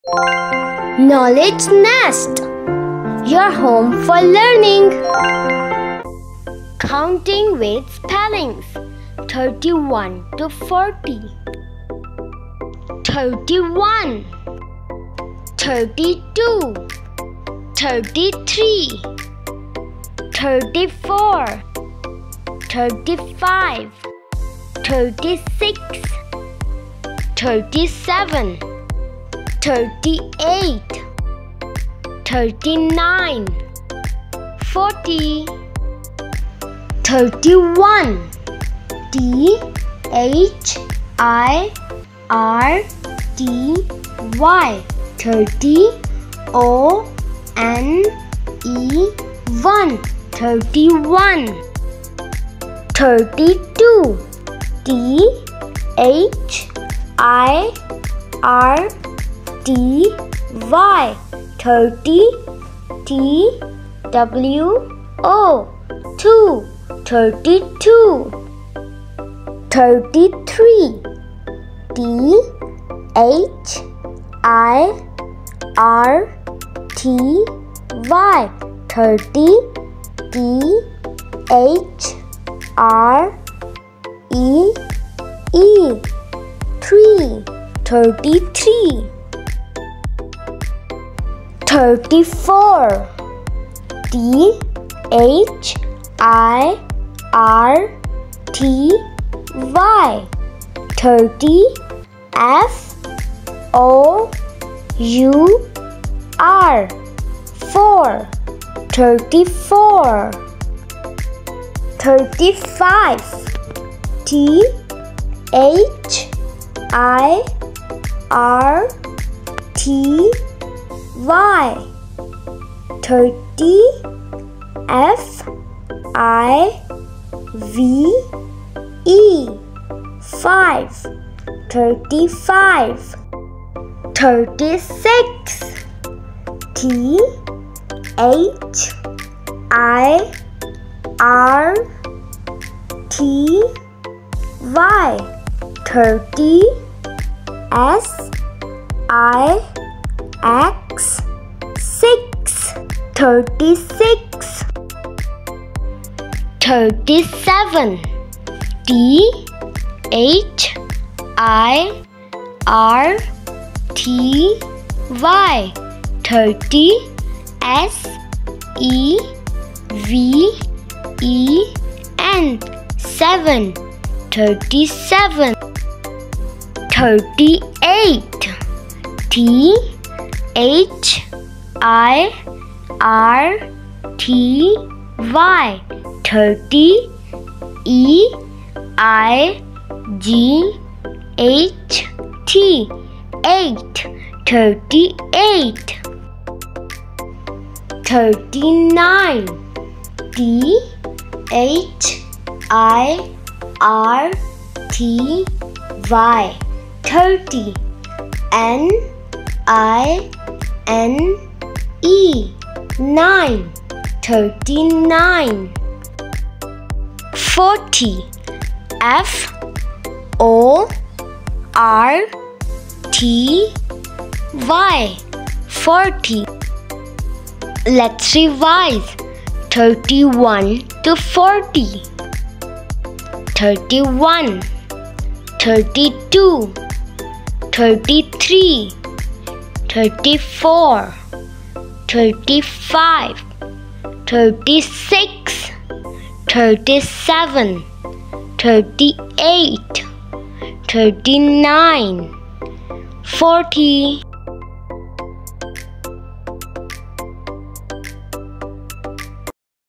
Counting 31 to 40 with Spelling | Learn Numbers for Kids | Number with Spellings Learning
Welcome to our fun learning video for kids! 🎉
In this video, children will learn counting from 31 to 40 along with the correct spelling of each number in an easy and enjoyable way. Perfect for preschool, kindergarten, and early learners!

What’s Inside:
✅ Counting numbers from 31 to 40
✅ Spelling each number clearly
✅ Bright visuals & fun animations
✅ Child-friendly voice and music

Benefits for Kids:
📚 Improves number recognition
🖋 Builds spelling skills
🎯 Enhances early math confidence

Whether your child is just starting school or wants to practice counting, this video is a great tool for learning at home or in the classroom.